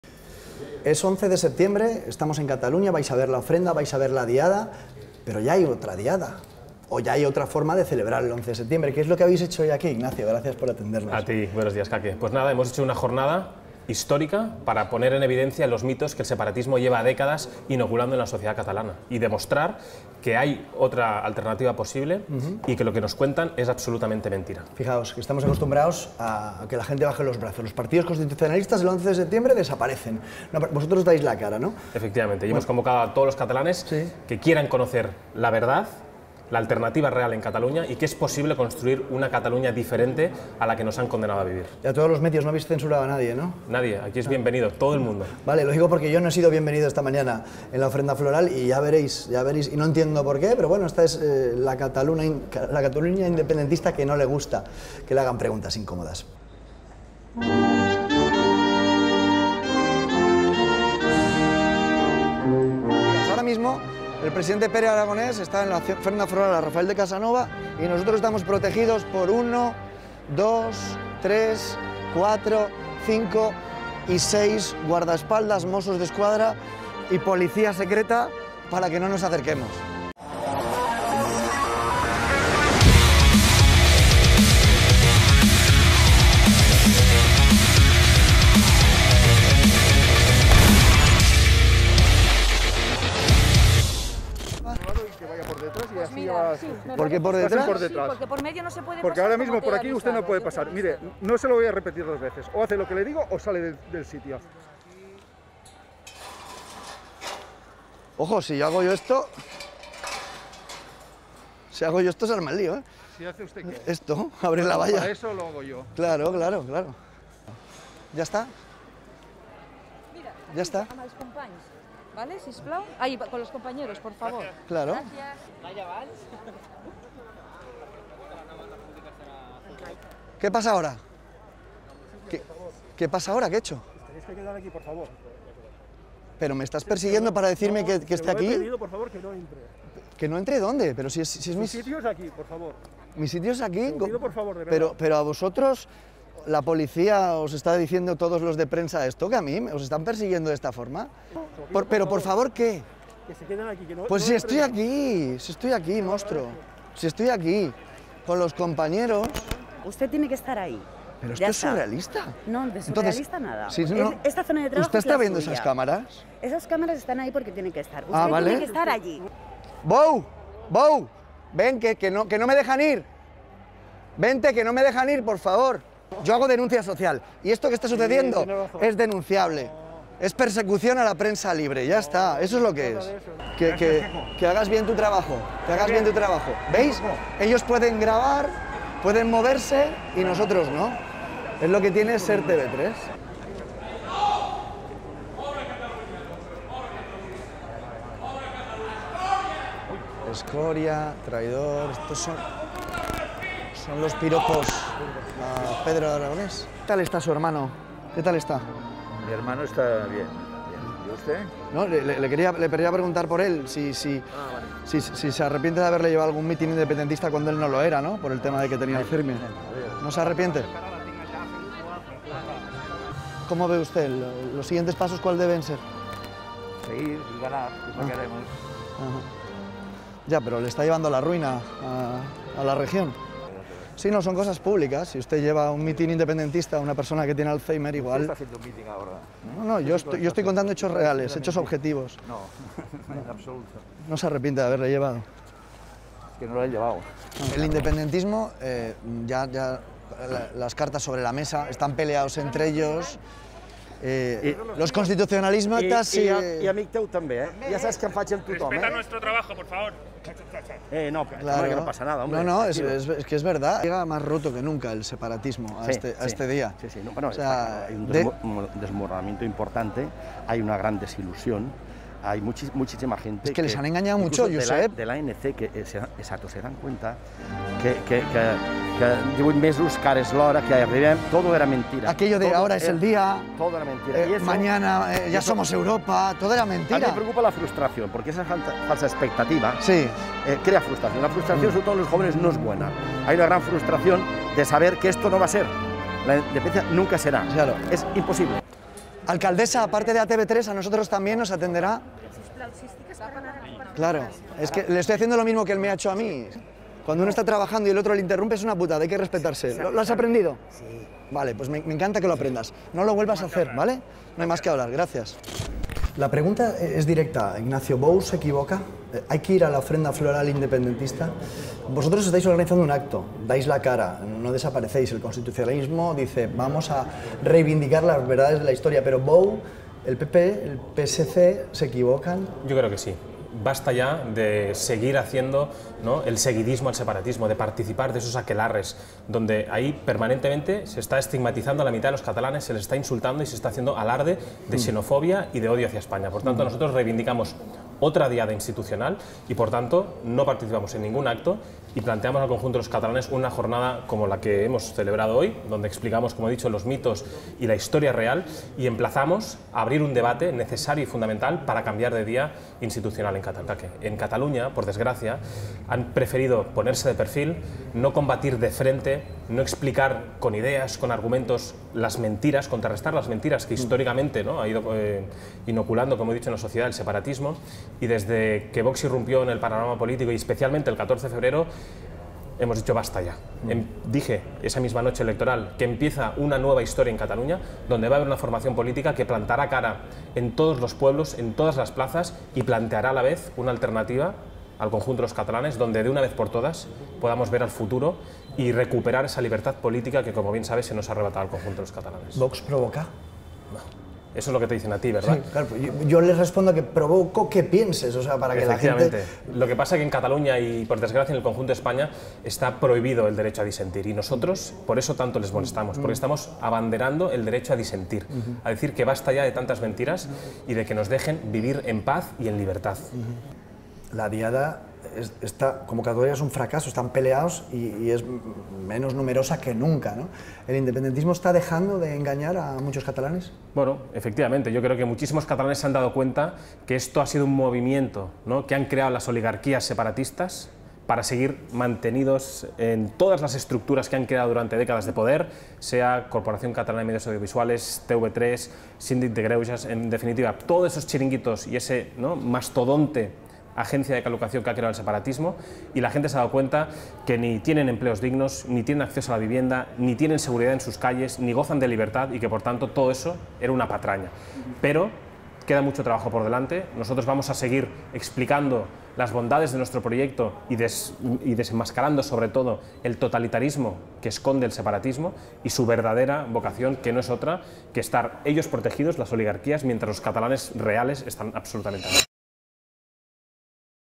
LAS DOS DIADAS - LOS DOS PERIODISMOS
Os presentamos las dos celebraciones de La Diada en Barcelona.

En primer lugar, entrevistamos a Ernest Maragall de ERC, a Josep Bou del PP y mostramos cómo las autoridades de la Generalidad nos pretenden impedir realizar nuestro trabajo y mientras, recibimos el acoso de un periodista de TV3.

Por otro lado, Ignacio Garriga nos muestra la Diada Constitucionalista, desmontando los cuentos del separatismo con una convocatoria en Barcelona a la que también hemos acudido.